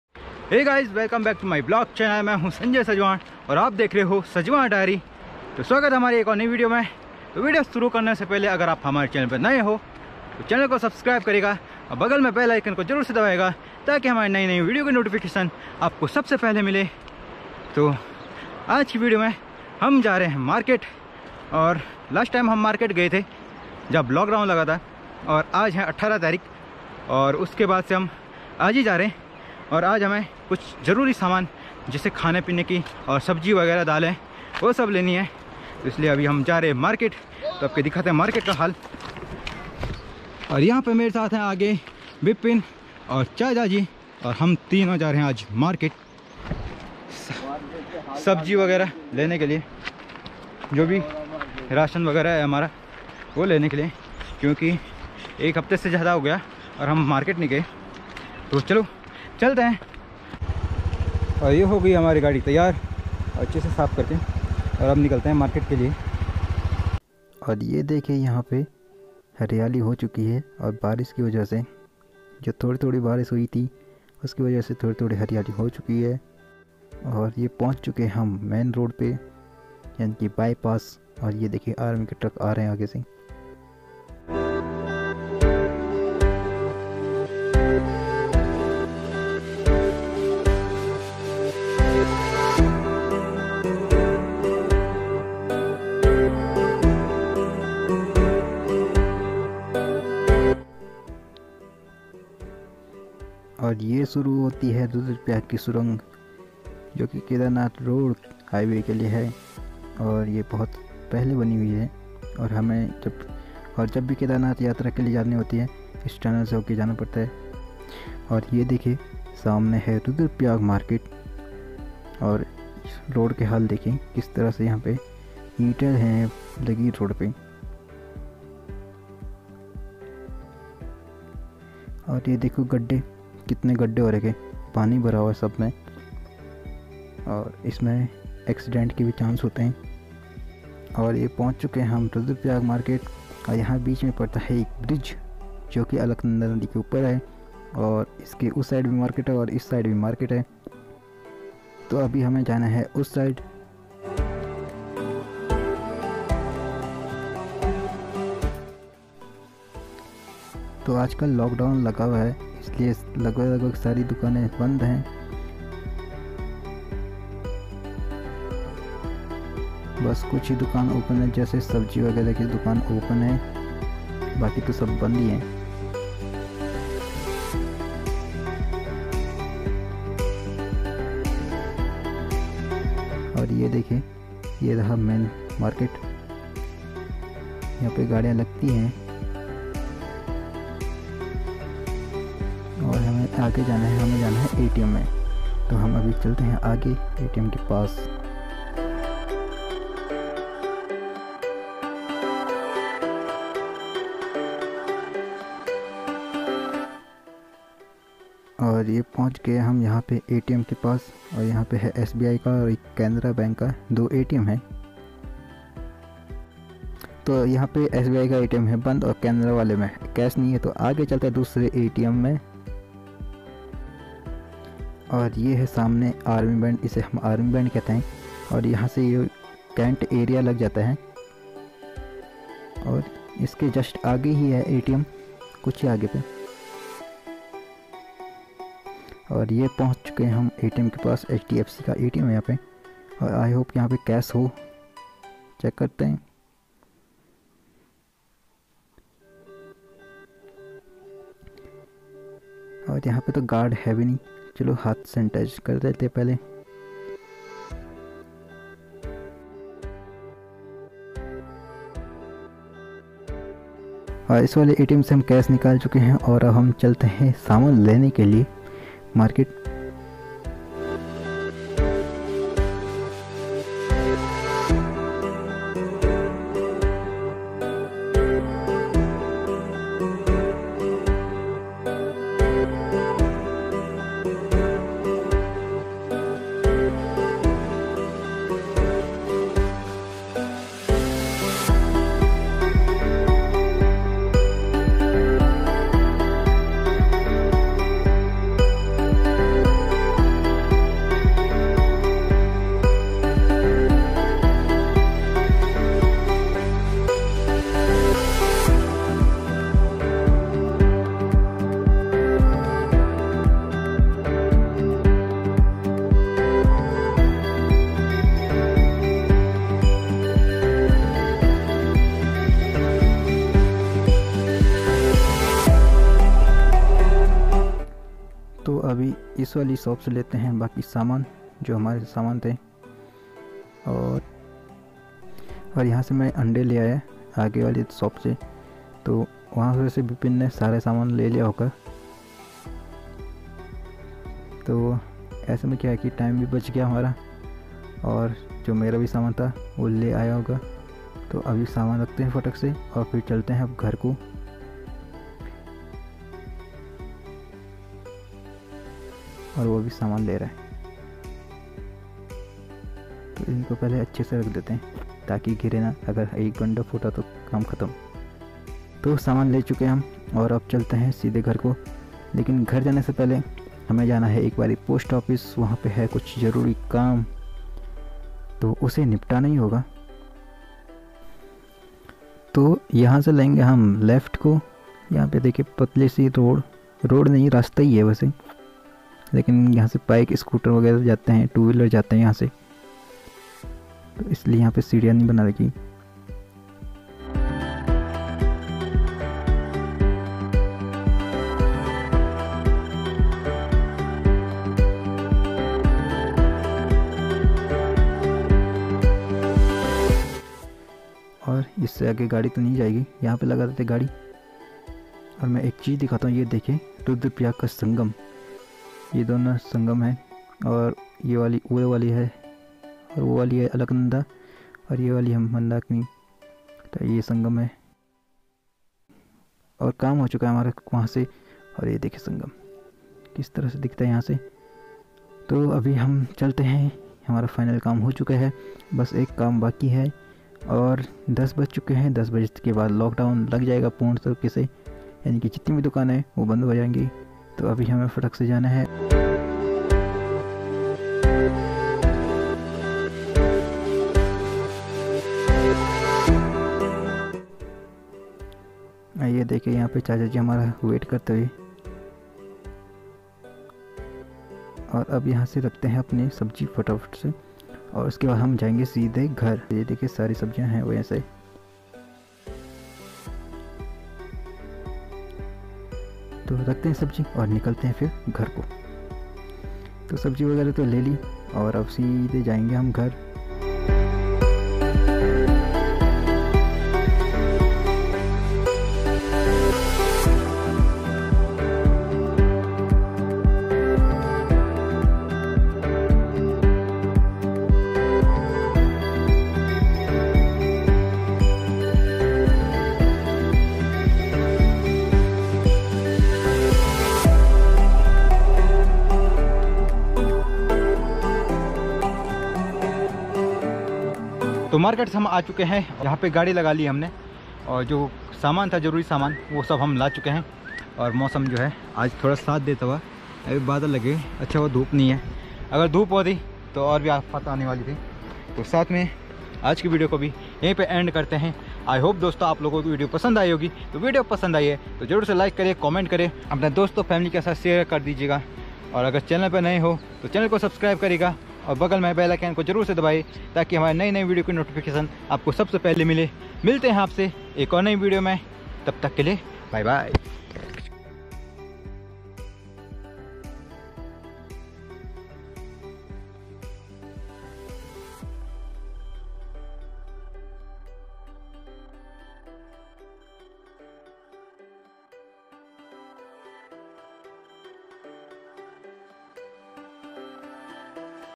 कैसा है Lockdown में रुद्रप्रयाग Market ? राशन खत्म हो गया 😢| Kyu Jana Pada Market | sanjay sajwan
Hello Friends Thanks for Your Love I Hope You Loved  This video  & keep Supporting

If u want contect me -